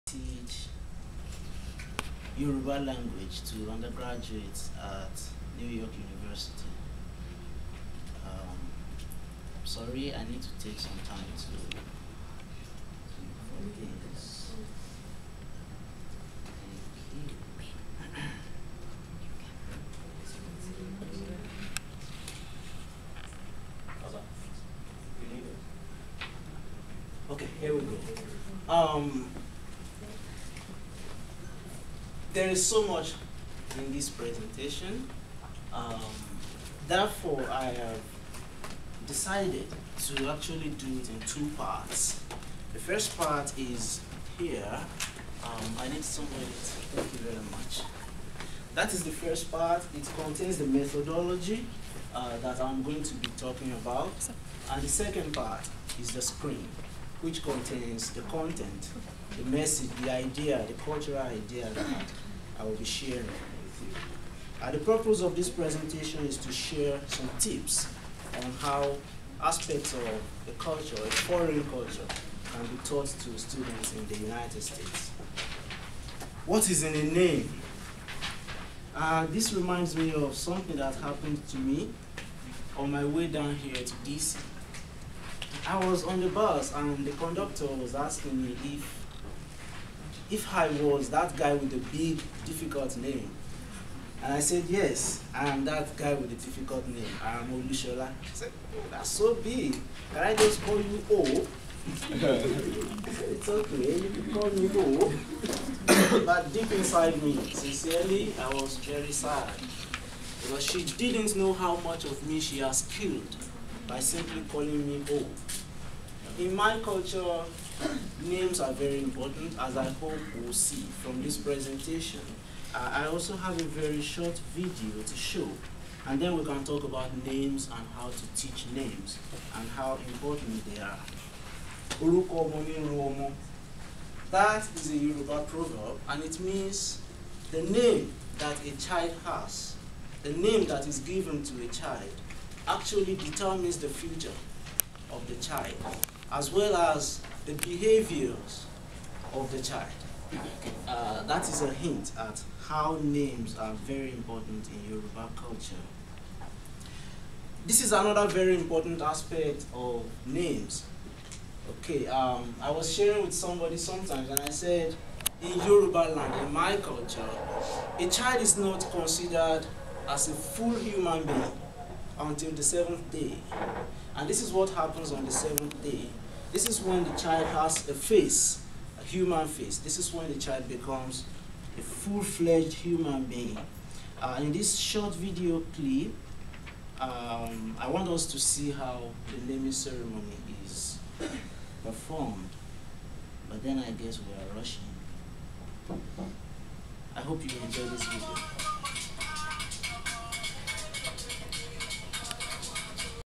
Naming in Yoruba: What's in a Name?
A lecture presented at the Fulbright Mid-year Conference at Washington DC by OYADIJI Olushola.